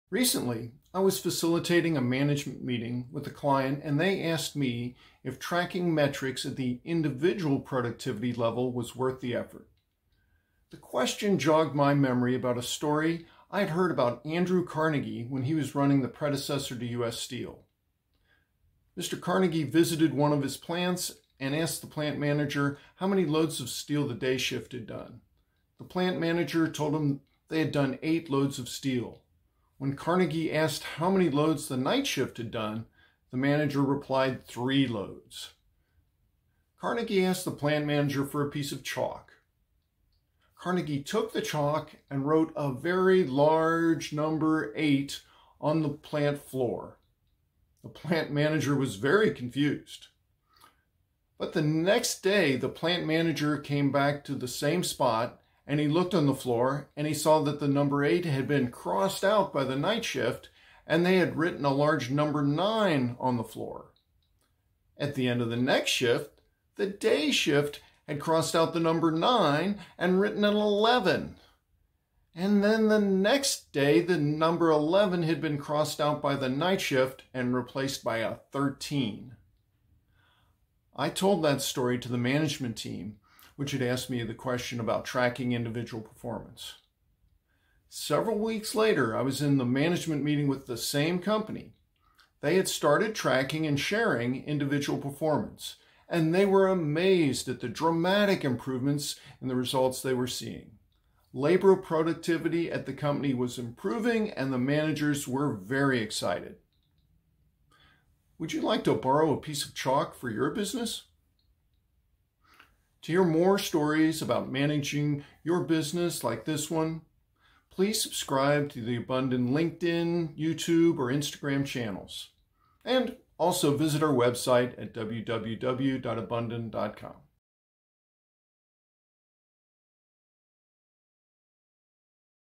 Can I Borrow a Piece of Chalk?
Do you think tracking metrics at the individual productivity level is worth the effort?

Watch my video to hear the story of how a very successful entrepreneur improved labor productivity.